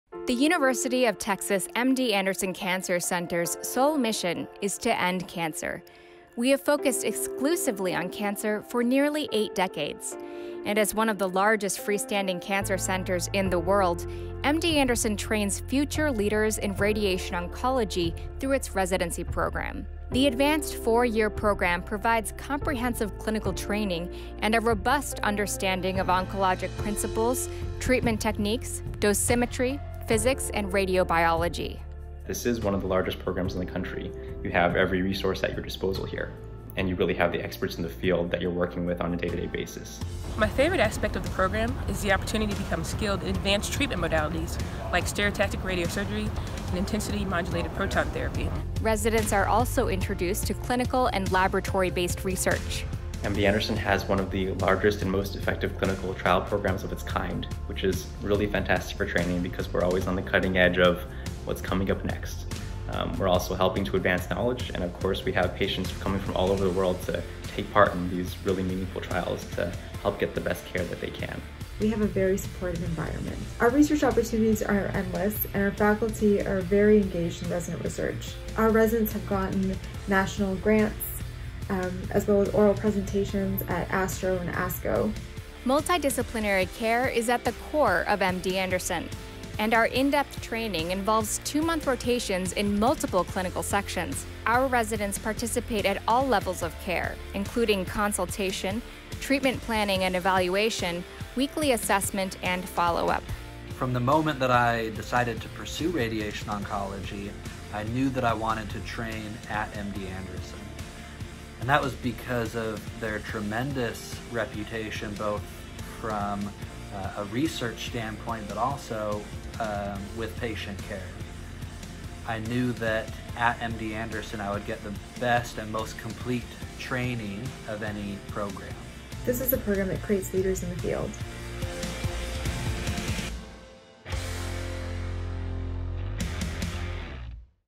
Radiation Oncology Residency at MD Anderson
Take a peek at radiation oncology residency life at The University of Texas MD Anderson Cancer Center and hear what a few of our residents have to say about their experience, the program and MD Anderson.